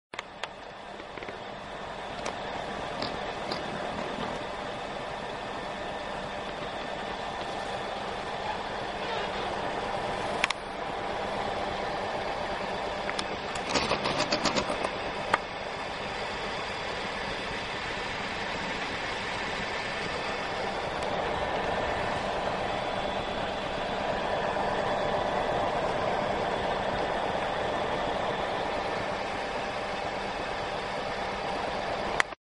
Clothes peg Mirror drum test run
Here was my Closes peg mirror drum test run .
Making it to view NBTV i found it very hard to adjust the mirror right due to the differences in the wooden pegs ...The mirror drum ended up as the Staggered mirror drum as i dropped the peg idea .
This took months to make while the staggered one took half an hour but they are what they are .
Its always fun to try something new and at the time i didn't know how to modulate the laser diode correctly i know now i should of by passed the electronics and just used the laser diode as the electronics will filter out the most of the video band width .
Learn form mistakes !